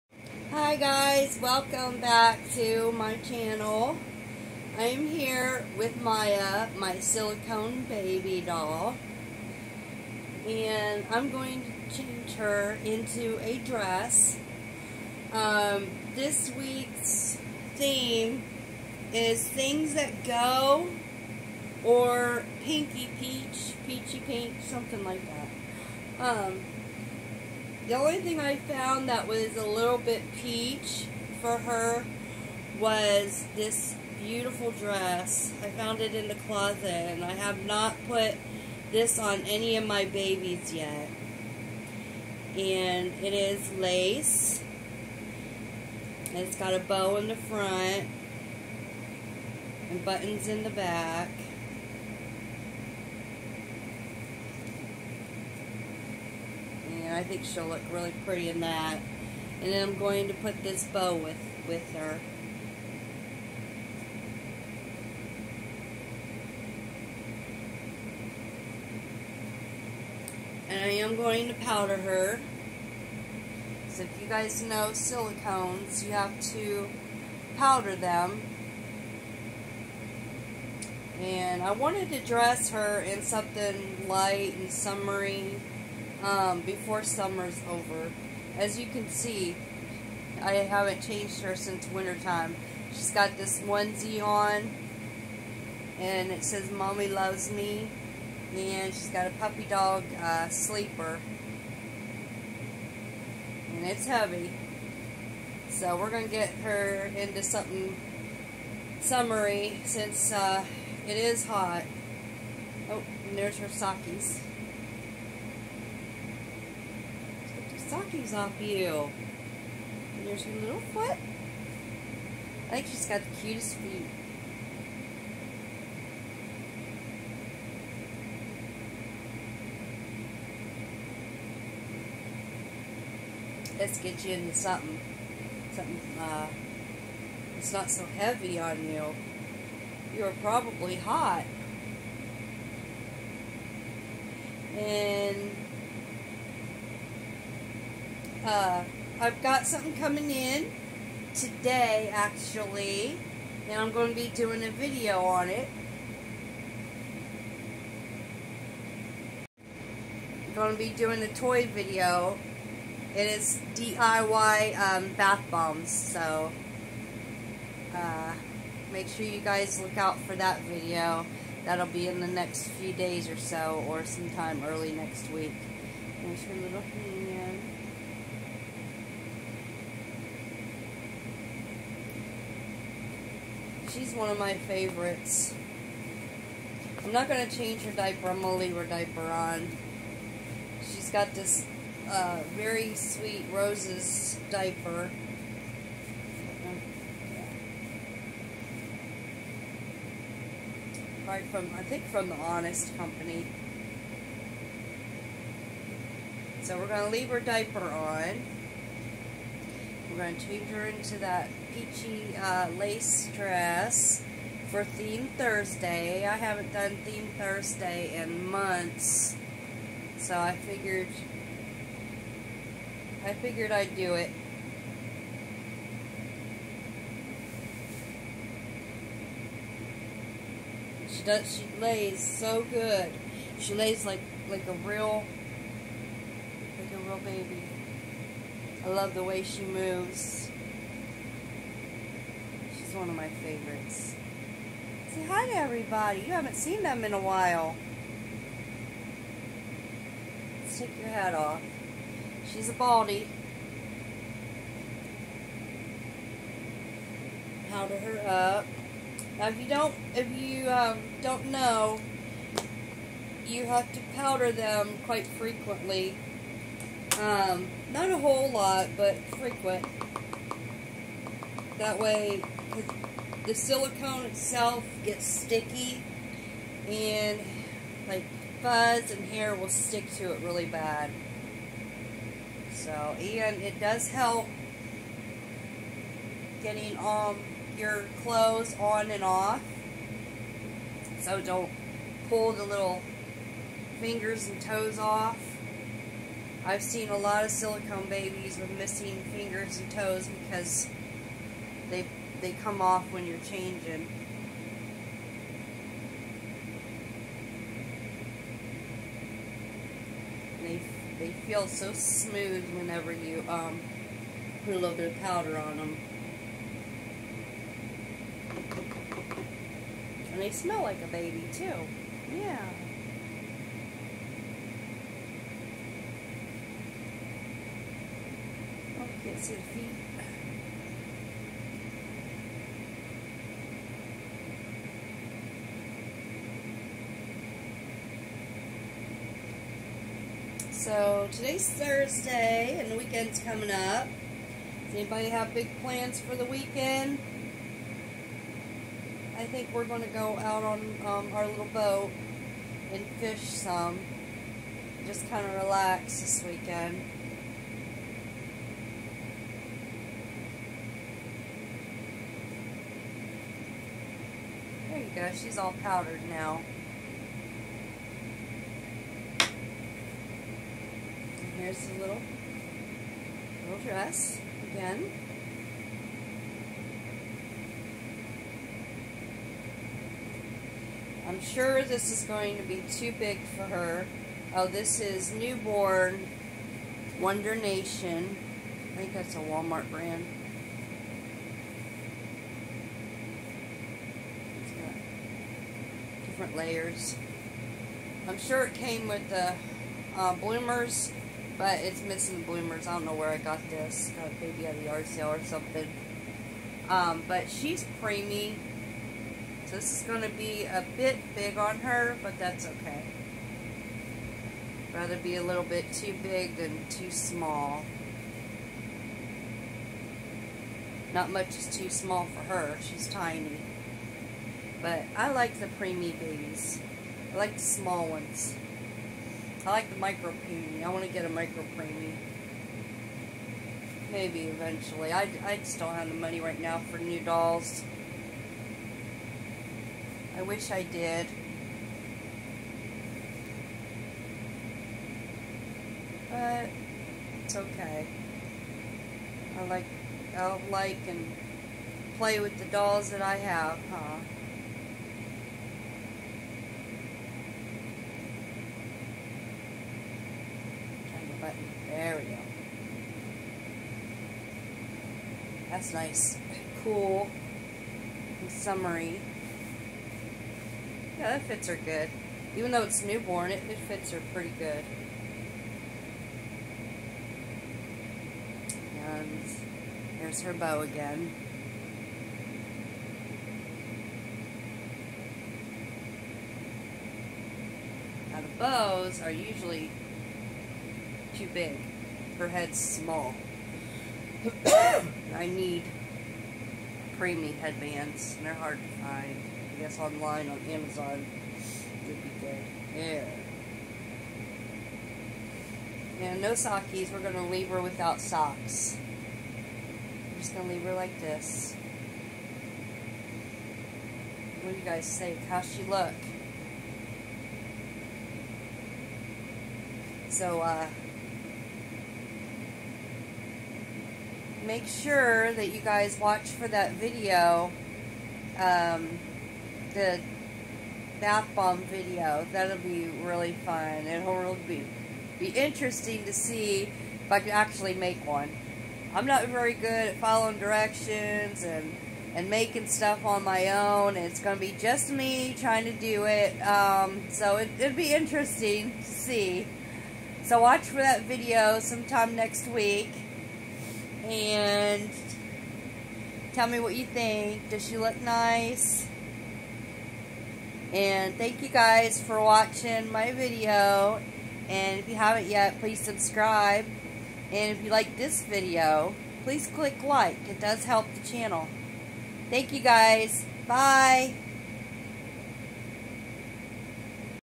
changing my SILICONE doll Theme Thursday
changing my silicone doll into a lace dress for theme Thursday

*videos for those that never grew up*